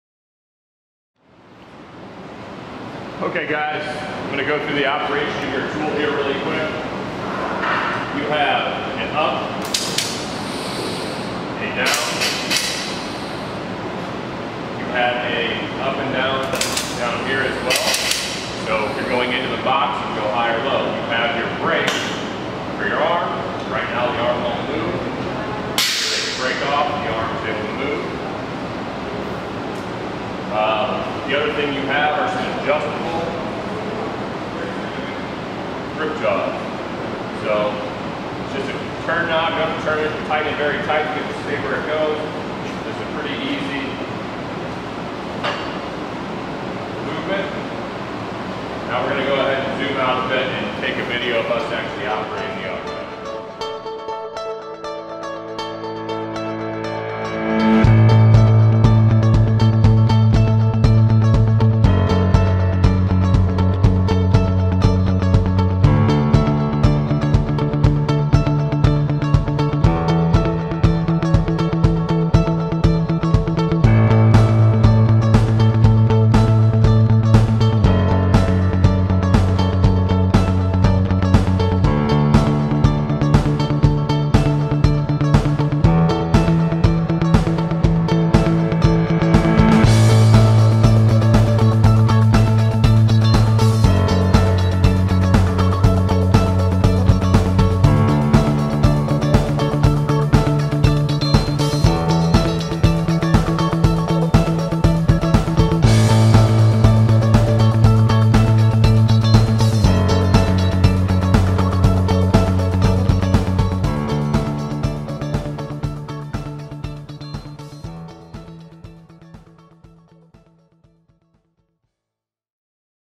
Easy Manipulator Arm (EMA) with Power Line Insulator Grip Tool
We integrated our Easy Manipulator Arm (EMA) with a custom lifting device to grip and lift large power line electrical insulators. The air powered Easy Manipulator Arm features: precision bearings for smooth, accurate 360-degree rotation at the mast, and 360-degree rotation at the secondary arm. The EMA comes equipped with operator-controlled brakes that prevent the arms movement at each rotation point. The grip tool features a steel and aluminum powder coated frame, ergonomic high/low handle bar with high/low -up/down controls, rotation brake controls, and adjustable grip jaws.